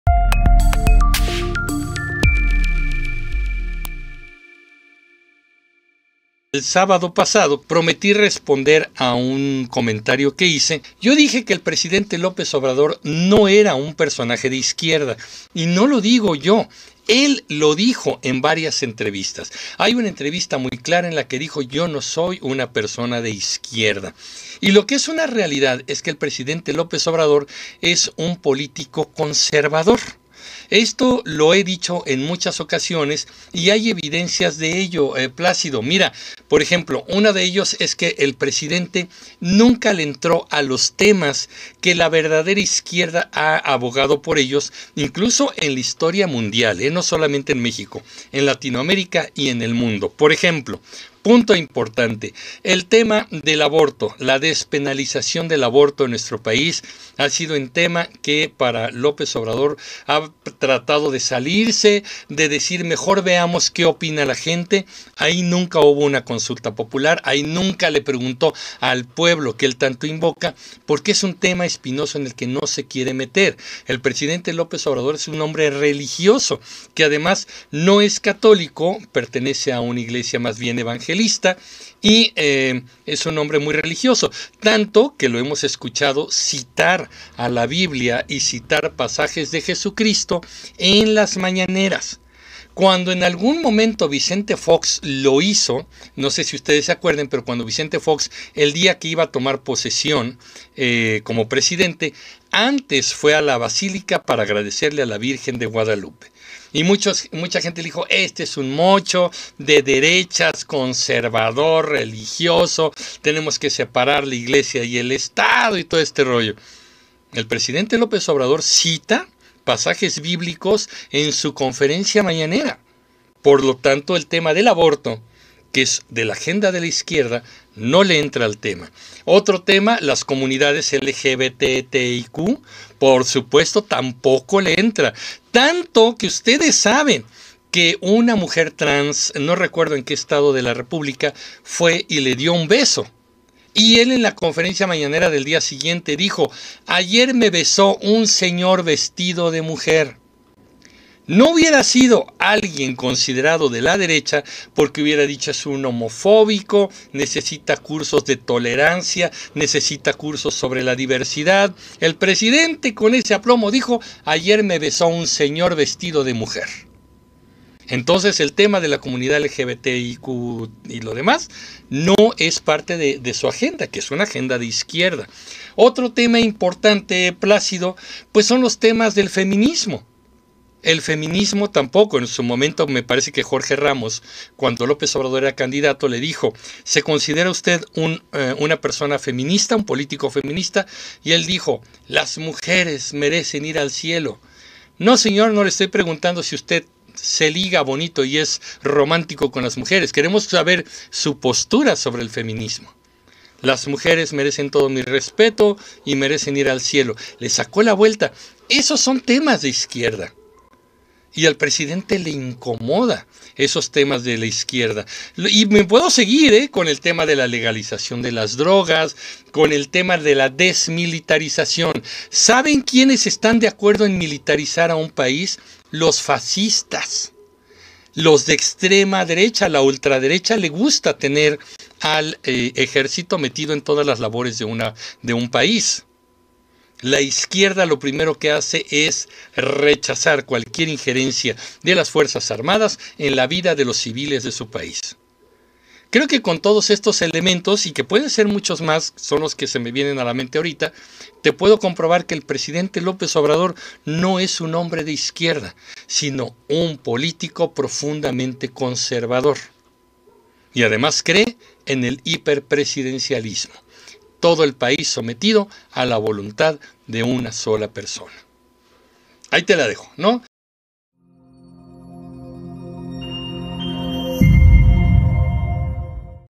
LÓPEZ OBRADOR NO ES DE IZQUIERDA, ES CONSERVADOR
Ante la falsa percepción de que López Obrador es un político de Izquierda, aquí doy algunos argumentos que comprueban que es profundamente conservador.